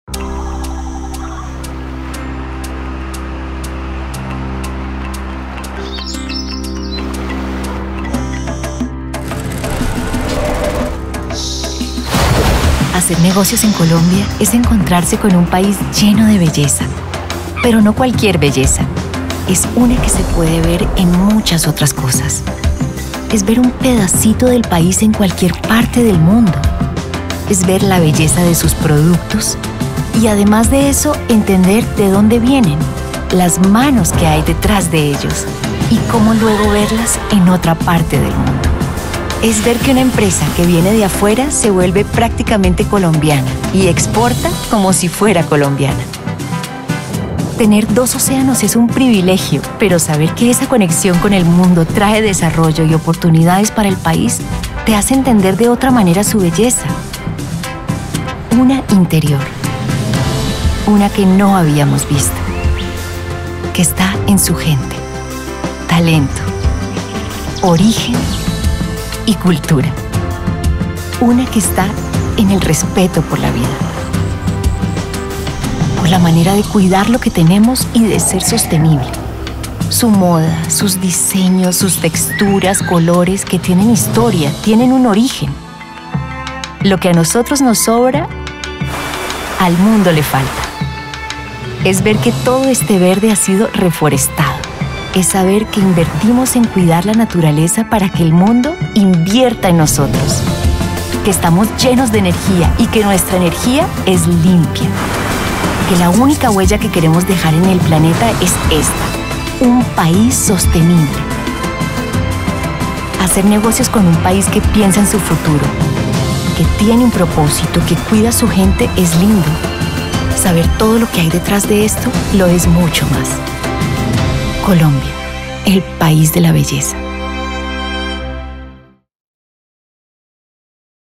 Colombia, ElPaisDeLaBelleza
Hacer negocios con nuestro país es reconocer el talento que está detrás de nuestros bienes y servicios de OrigenColombiano. Es invertir en un país que le apuesta a la sostenibilidad, talento y conservación de la biodiversidad, a la vez que reconoce las tradiciones de sus regiones.

Créditos
- Los videos contienen imágenes del archivo de Señal Memoria de RTVC
- Imágenes extraídas de la película Colombia Magia Salvaje, producida por Grupo Éxito, Ecoplanet y OTF
- Agradecimientos a Fundación Ecoplanet